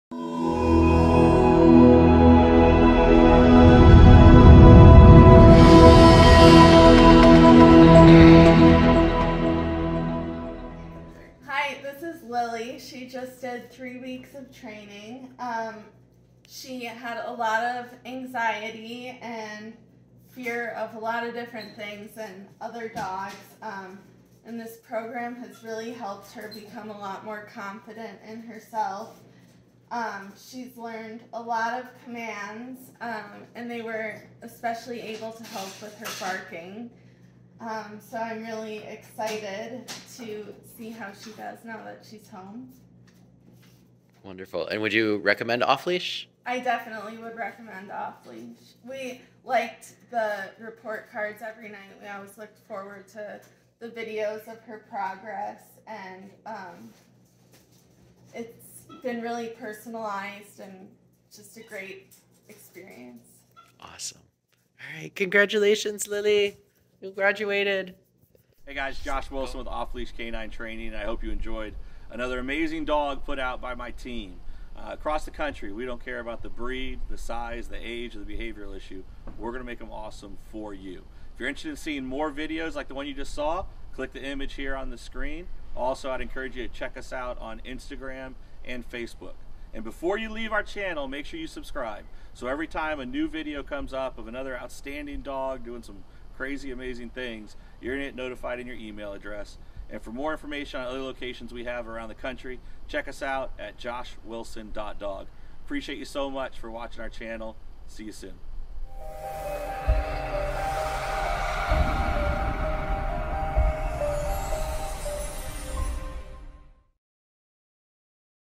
1 Y/O Chihuahua “Lily” / Owner Testimonial / E-Collar Training
Meet Lily, the adorable 1-year-old Chihuahua who has embarked on an incredible journey of transformation with the help of E-Collar training. In this heartwarming video, Lily's owner shares their testimonial about the positive impact E-Collar training has had on Lily's behavior and obedience.